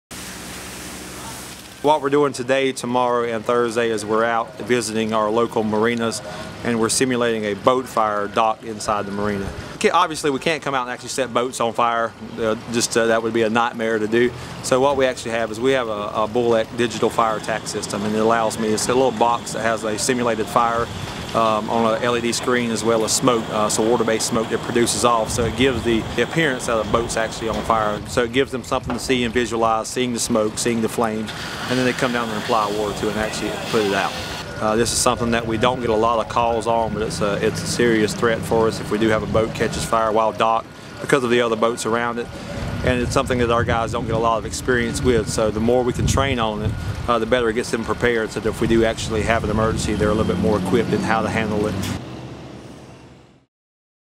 Marina Fire Training in NMB
The North Myrtle Beach Fire Department conduct training exercises at four marinas in North Myrtle Beach. The department is using a "BullEx Digital Fire Attack Training System", which creates a simulated flame and smoke. The exercises will continue Aug. 25-27th.